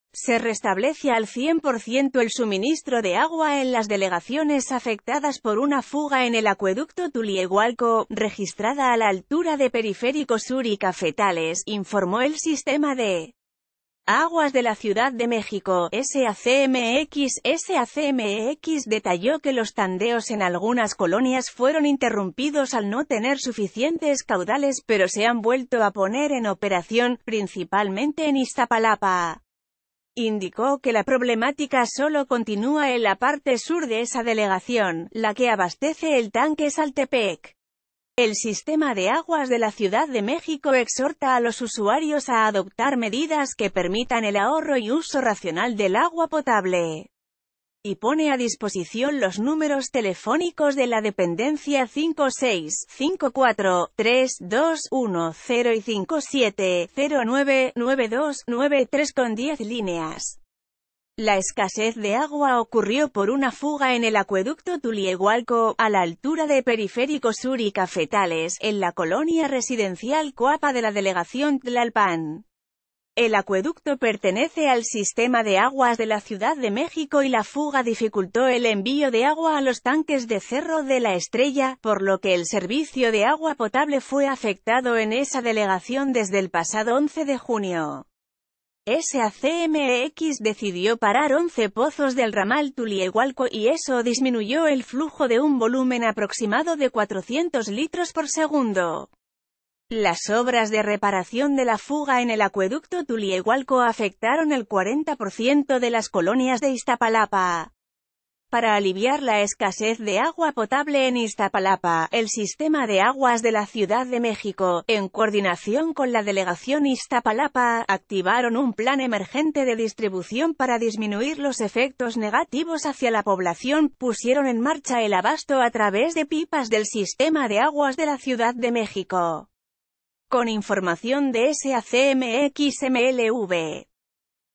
Restablecen suministro de agua potable en la CDMX
Se restablece al cien por ciento el suministro de agua en las delegaciones afectadas por una fuga en el Acueducto Tulyehualco, registrada a la altura de Periférico Sur y Cafetales, informó el Sistema de Aguas de la Ciudad de México (SACMEX).SACMEX detalló que los tandeos en algunas colonias fueron i...